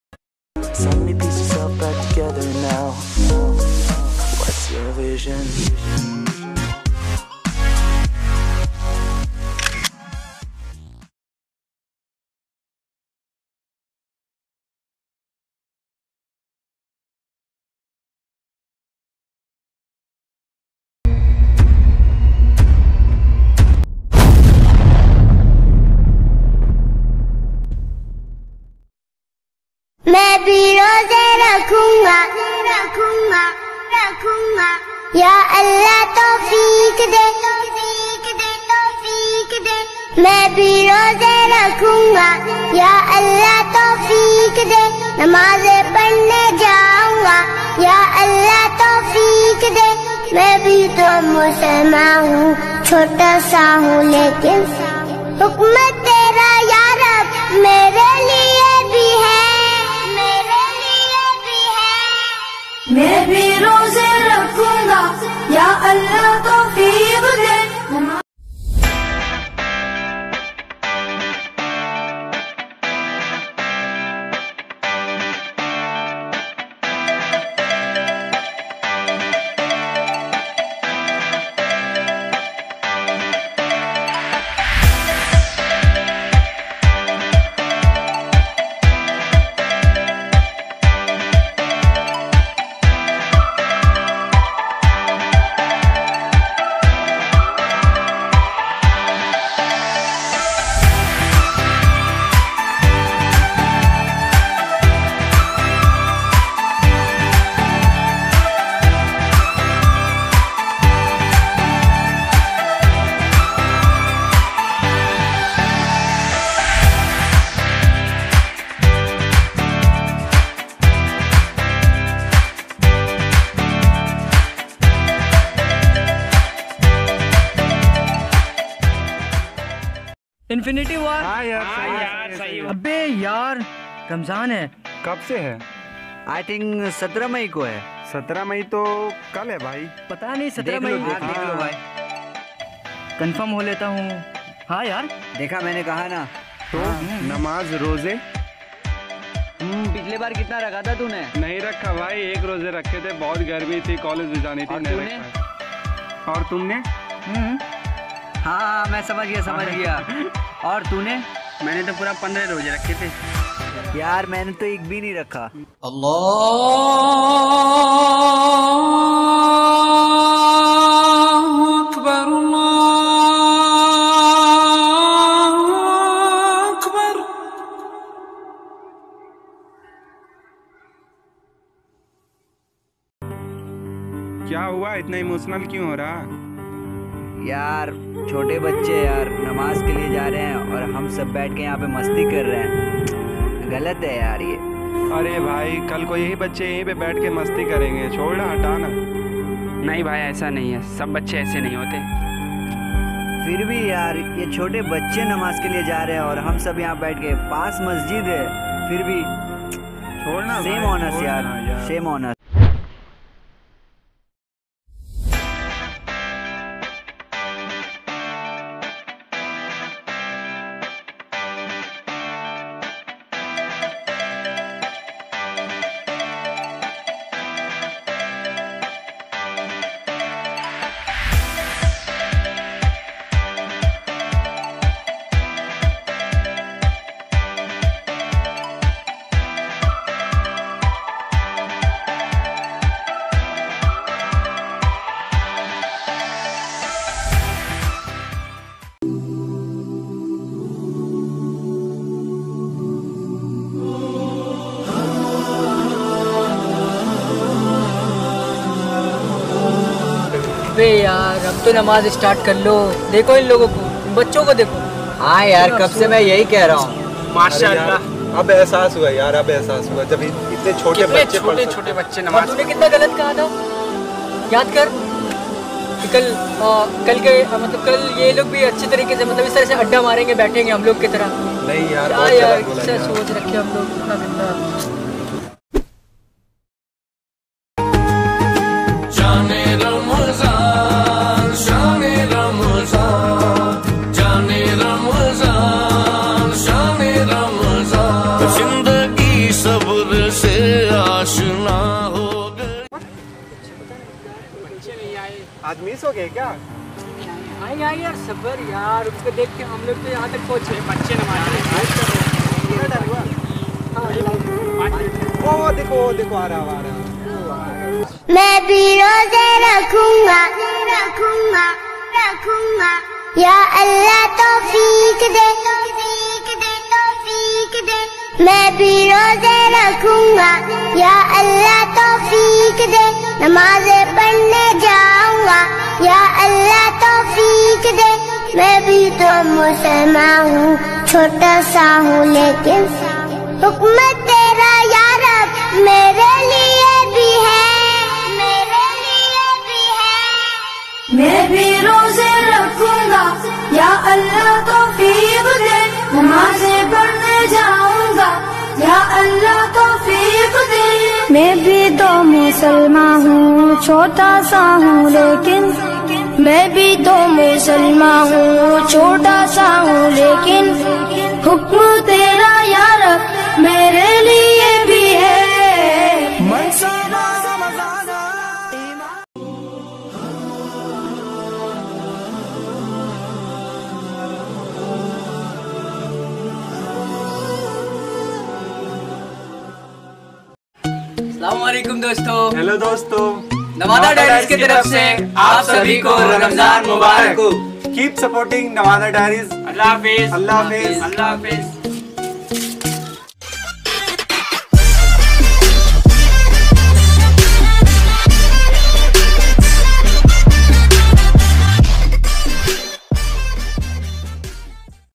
SHAN - E - RAMZAAN || RAMZAAN MUBARAK || RAMZAAN SPECIAL
Hi Guys. 😉I am SARTAJ From the channel [ Nawada Diaries ] Today I am Back With a new Video Based On Ramzaan Facts Welcome To my new video.

तो उम्मीद करता हु आपको ये Video पसंद आएगी.. :-)

SHAN - E - RAMZAAN
Directed - Sartaj Ansari
Story - Awesome Ansari ( Shahid)
Videography - Sartaj Ansari

Cast :-
Stylish Sartaj
S. Awesome Ansari
Sameer Zahid
Afsar Alam

Send Me Your Talent ❤